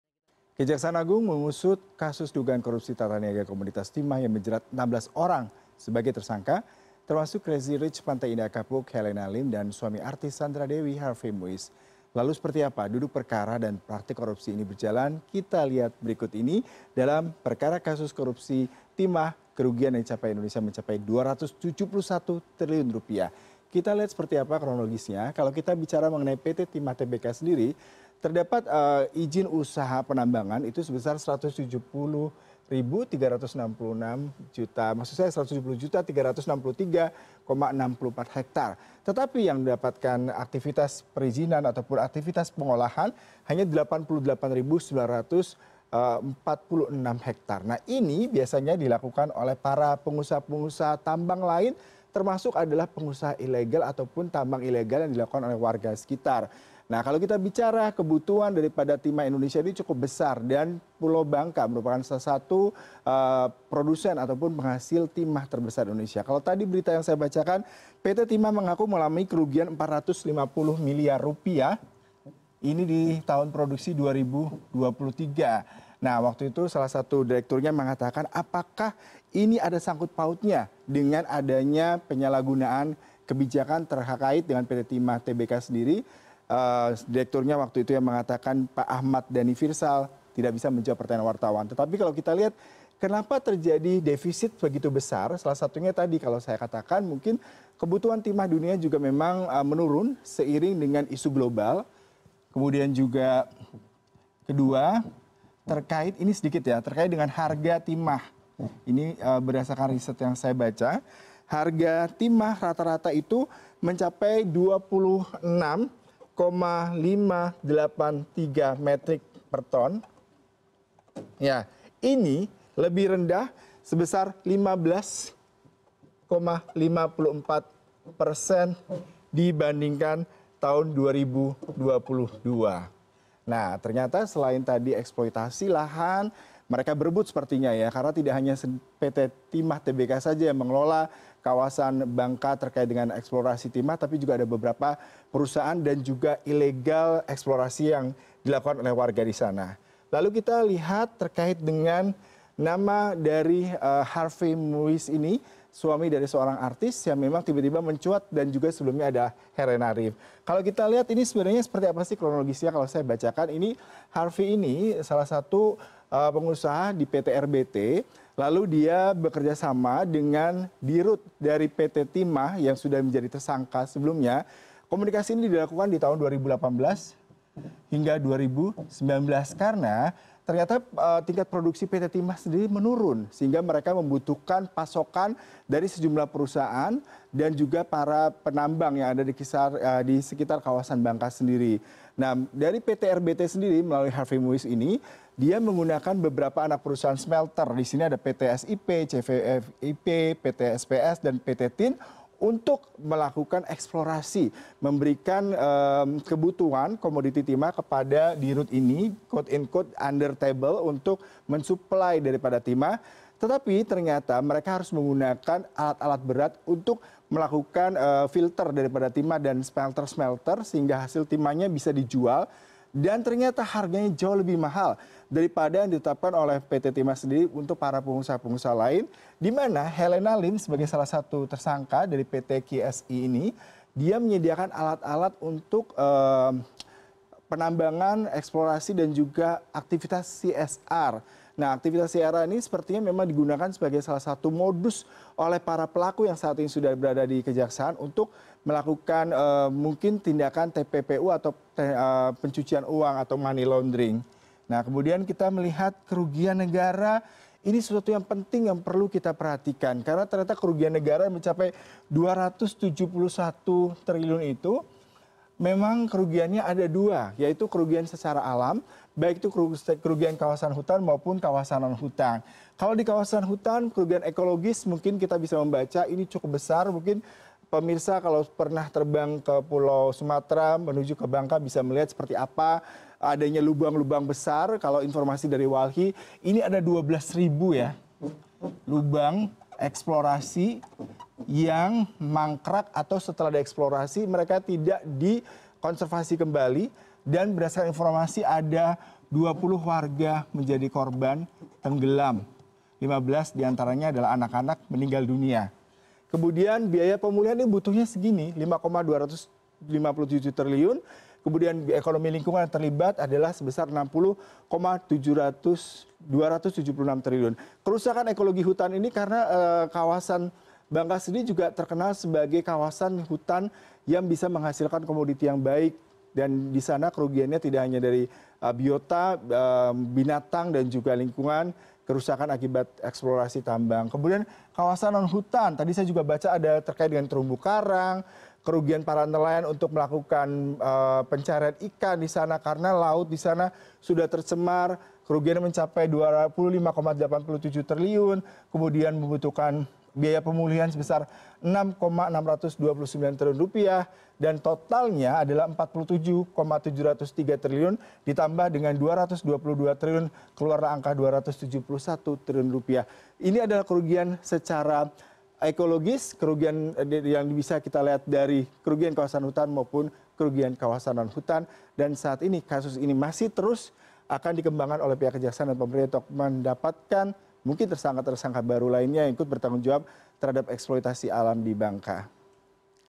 Duduk Perkara Kasus Korupsi Timah Rp271 T
Kejaksaan Agung mengusut kasus dugaan korupsi tata niaga komoditas Timah yang menjerat 16 orang sebagai tersangka, termasuk Crazy Rich Pantai Indah Kapuk Helena Lim dan suami artis Sandra Dewi, Harvey Moeis. Lalu seperti apa duduk perkara atau praktik  korupsi ini berjalan?